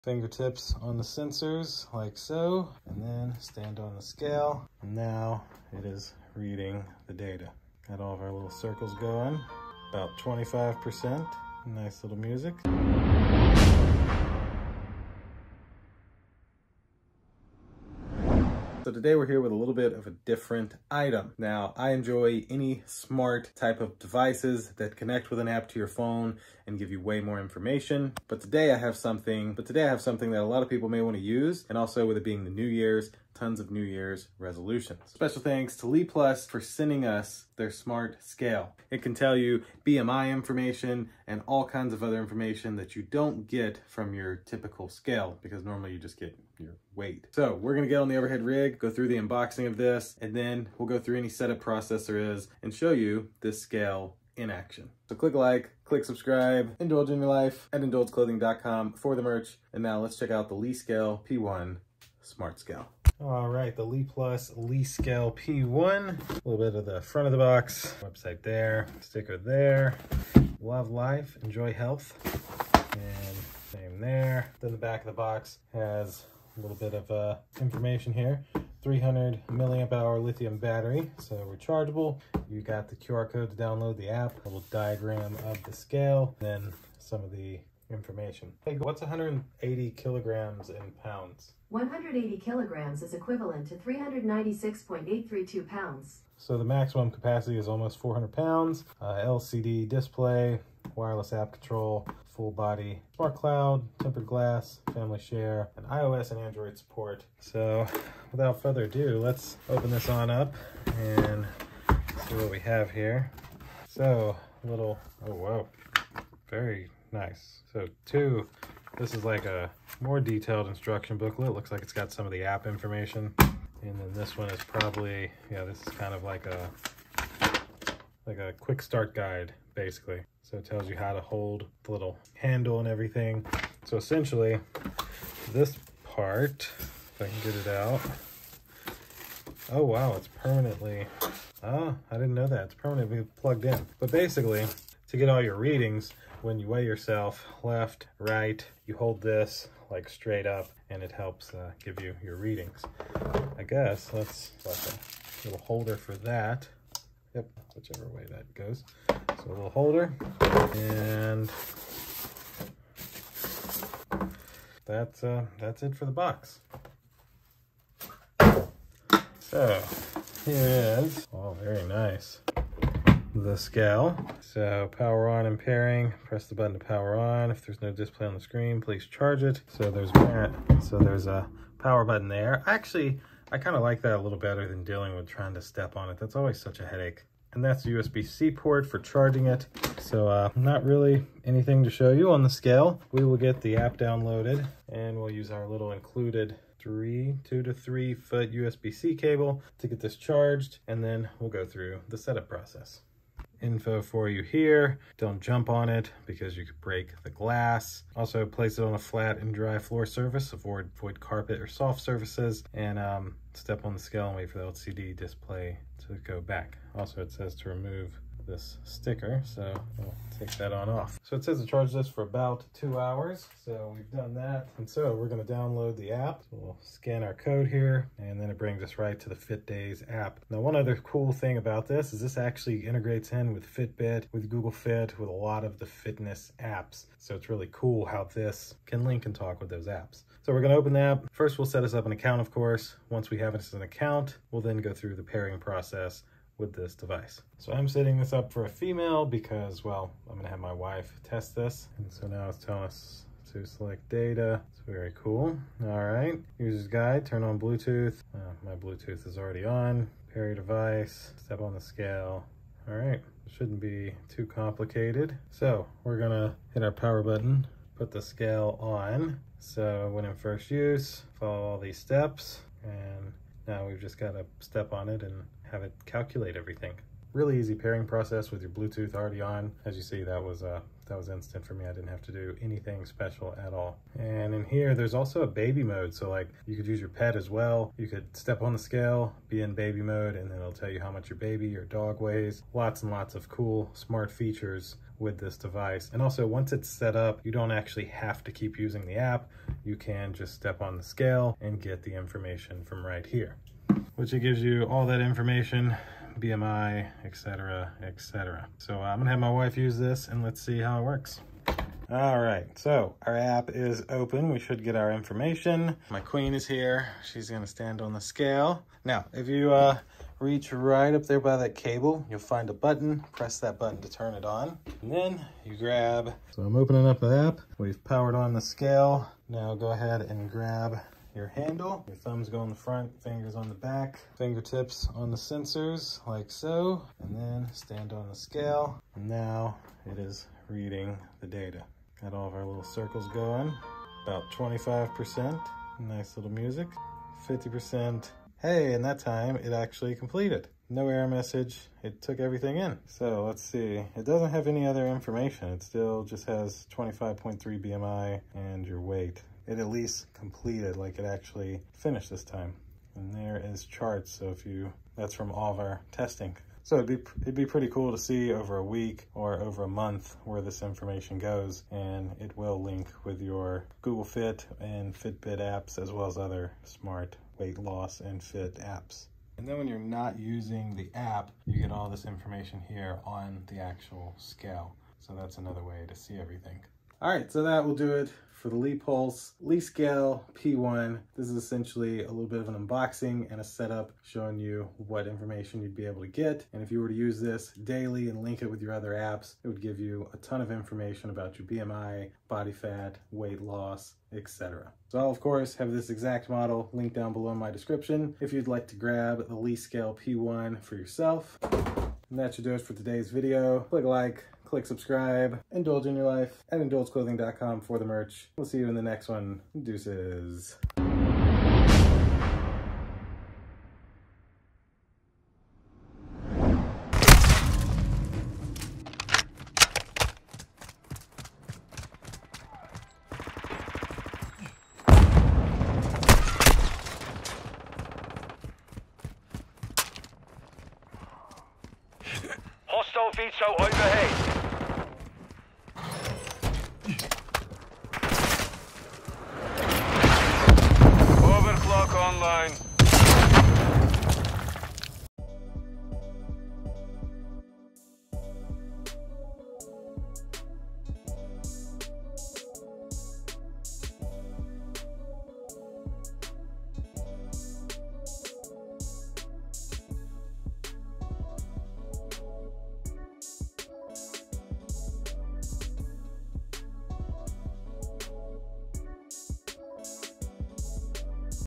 Lepulse Lescale P1 Smart Scale Review
Today we check out the Lepulse Lescale P1. This is a Smart Scale that links to your smartphone and so you can view your weight records, BMI and more. Check it out!

Do you want a Lepulse Lescale P1 Smart Scale?